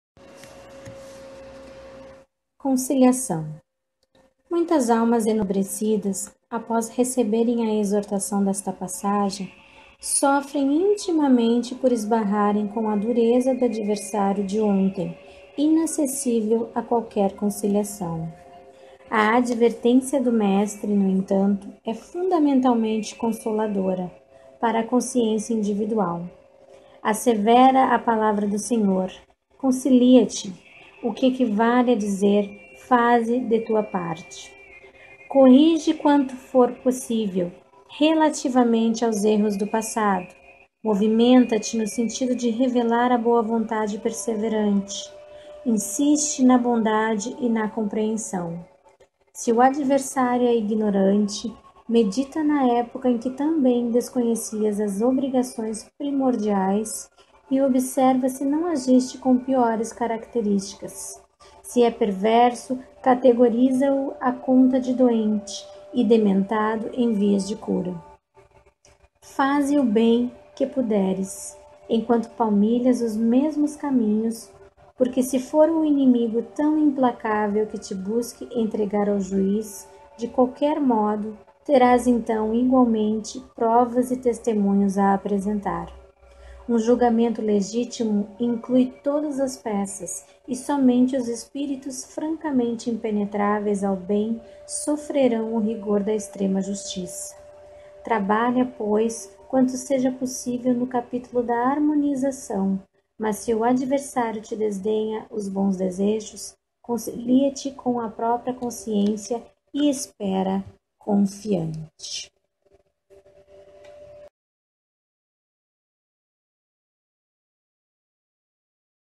Conciliação
Concilia-te depressa com o teu adversário,
enquanto estás no caminho com ele, para que não
aconteça que o adversário te entregue
ao juiz, e o juiz te entregue
ao oficial de justiça, e te encerrem na prisão.
- Jesus (Mateus, 5:25.)
Pão Nosso | Chico Xavier | Pelo Espírito Emmanuel | Página 265